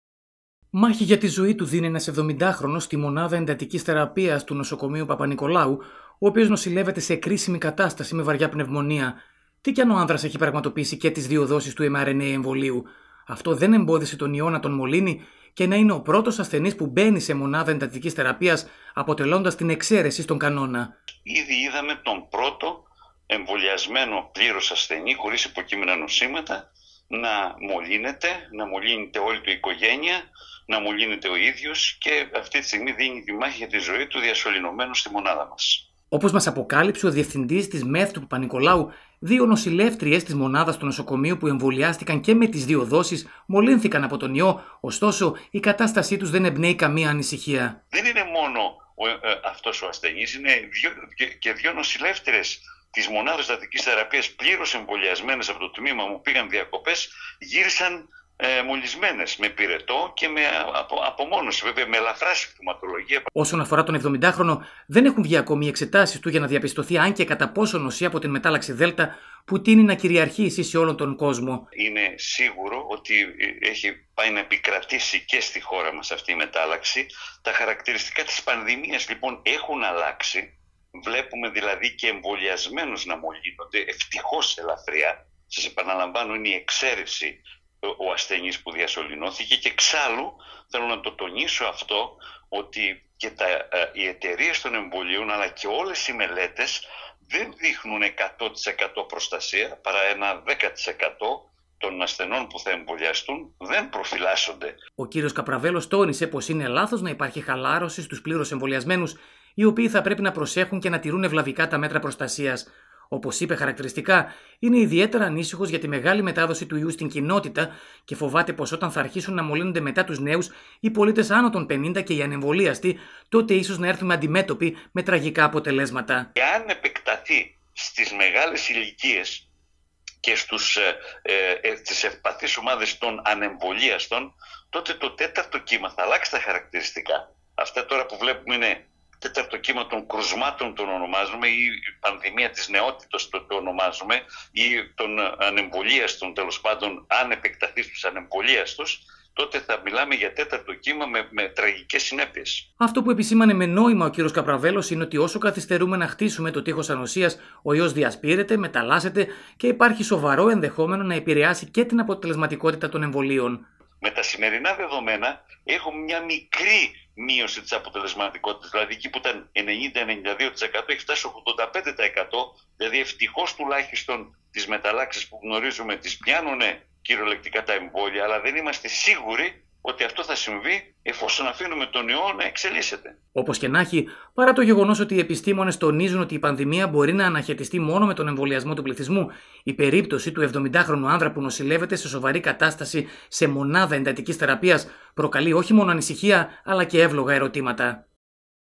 Στη ΜΕΘ ο πρώτος πλήρως εμβολιασμένος
Στη ΜΕΘ του Νοσοκομείου Παπανικολάου της Θεσσαλονίκης νοσηλεύεται ο πρώτος ασθενής, ο οποίος έχει εμβολιαστεί και με τις 2 δόσεις MRNA εμβολίου. Πρόκειται για έναν 70χρονο, χωρίς υποκείμενα νοσήματα, ο οποίος είναι σε κρίσιμη κατάσταση με βαριά πνευμονία. Να σημειωθεί ότι και 2 νοσηλεύτριες της ΜΕΘ του Παπανικολάου, που έκαναν και τις 2 δόσεις του εμβολίου, μολύνθηκαν, ωστόσο νοσούν με ήπια συμπτώματα.